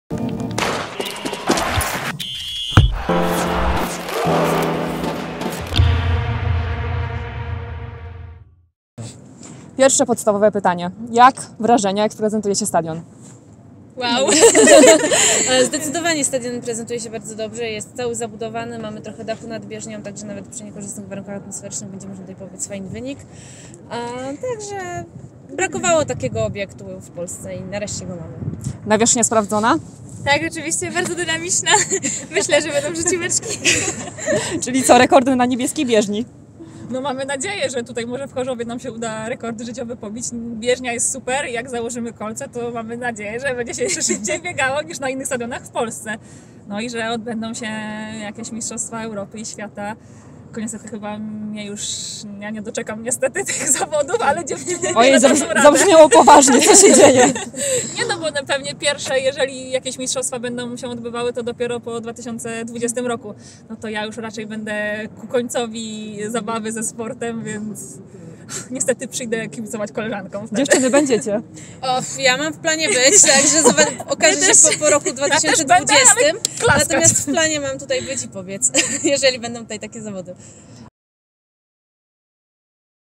Patrycja Wyciszkiewicz, Aleksandra Gaworska, Iga Baumgart o Stadionie Śląskim
Wypowiedź Patrycji Wyciszkiewicz, Aleksandy Gaworskiej oraz Igi Baumgart po konferencji prasowej 18 września 2017 roku.
Archiwum Stadion Śląski Sp. z o.o.